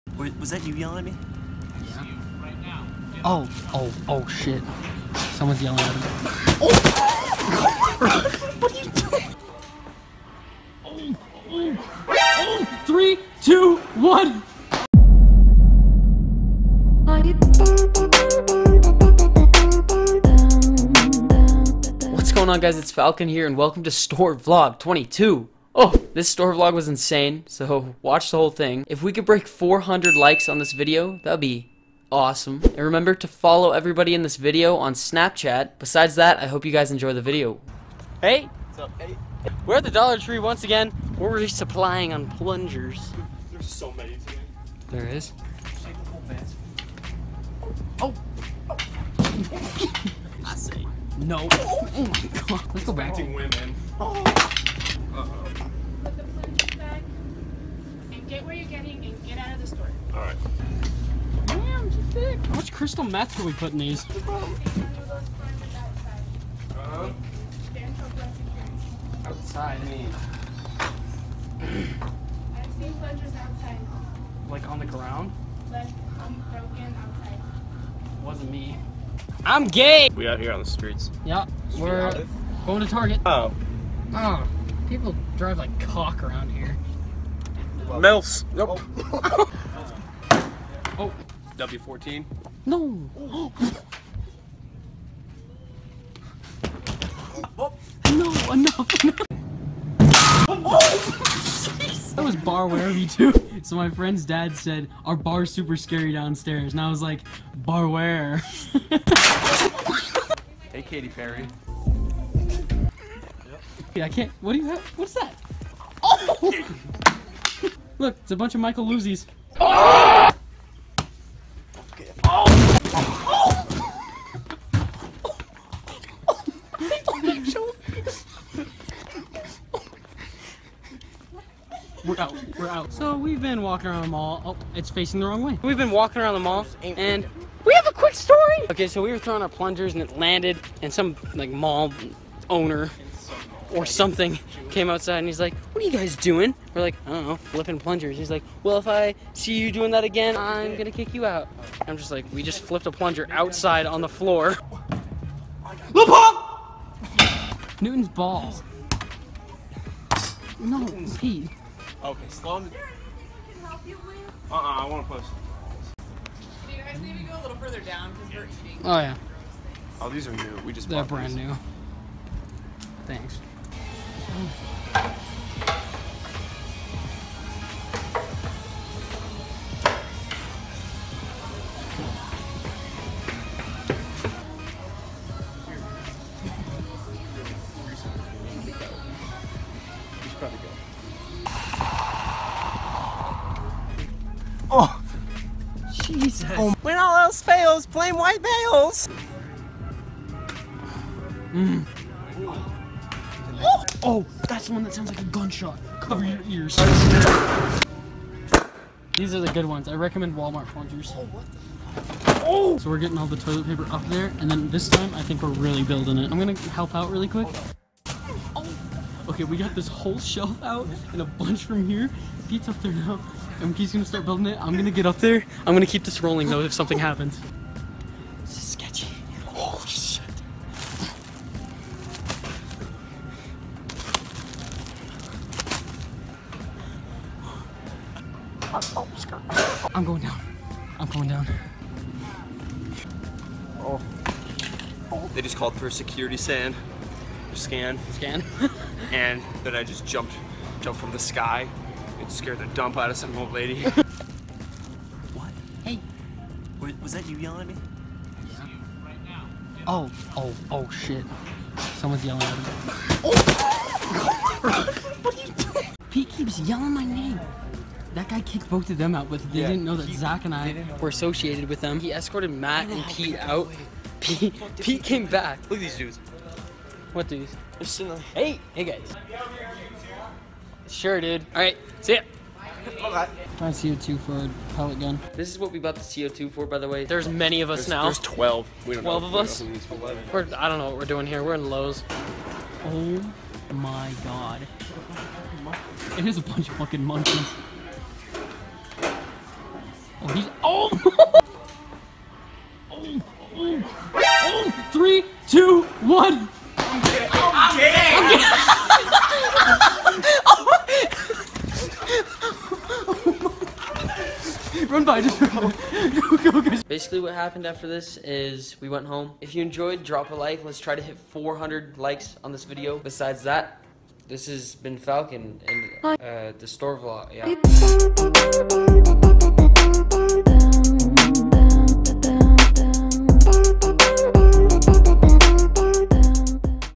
We ACTUALLY Built a Toilet Paper Fort... ON TOP OF WALMART! (Store Vlog #22)
I hope you enjoyed this video!!! 400 Likes?!?!